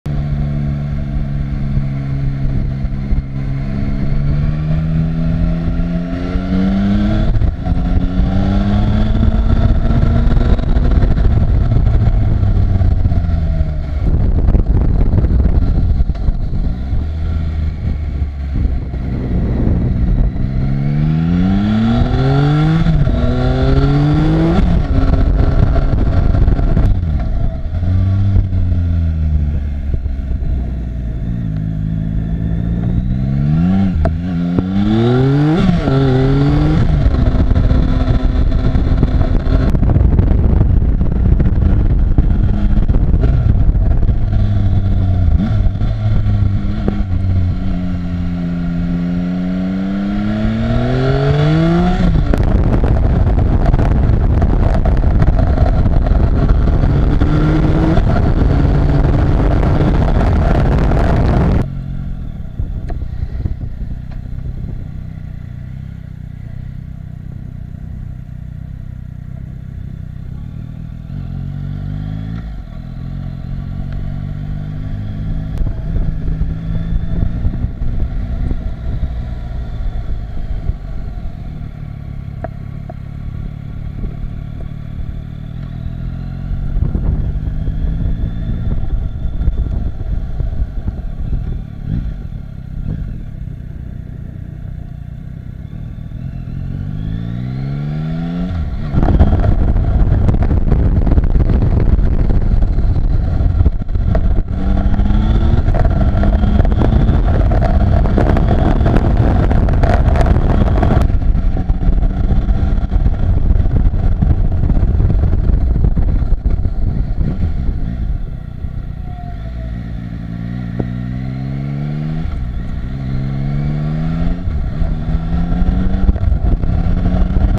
Test drive Yamaha Tracer 900
moto perfetta, giusto equilibrio tra potenza peso e guidabilità.